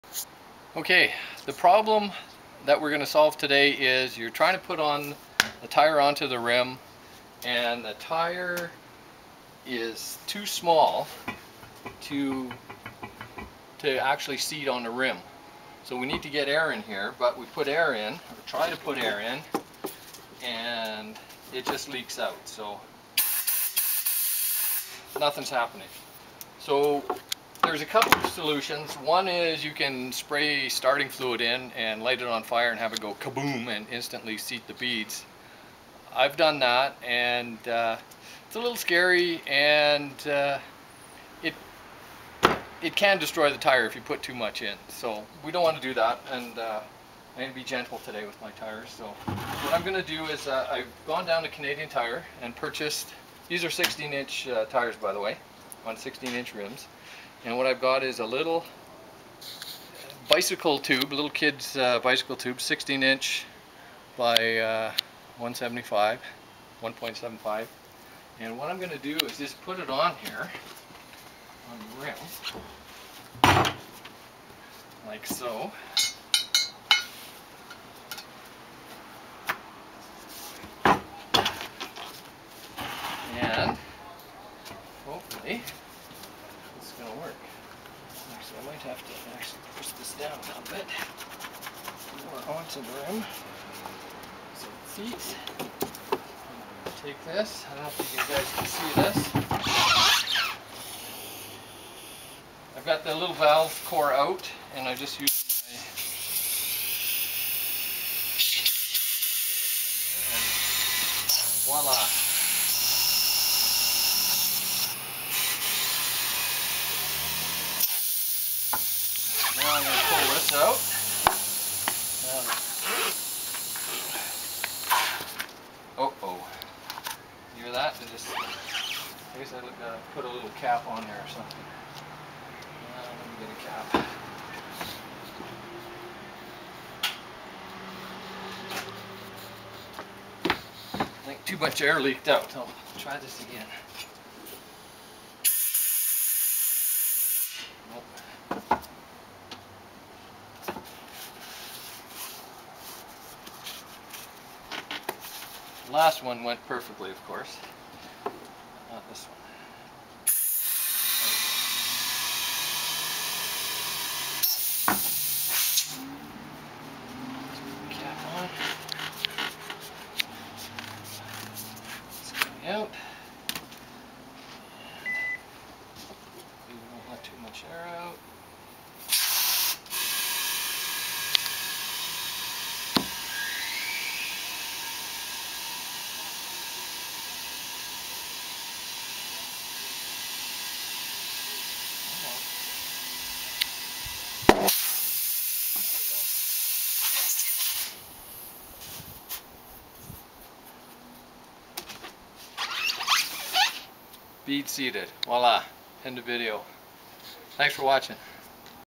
Seating tire on rim
Getting the tire bead to seat on the rim without using ether.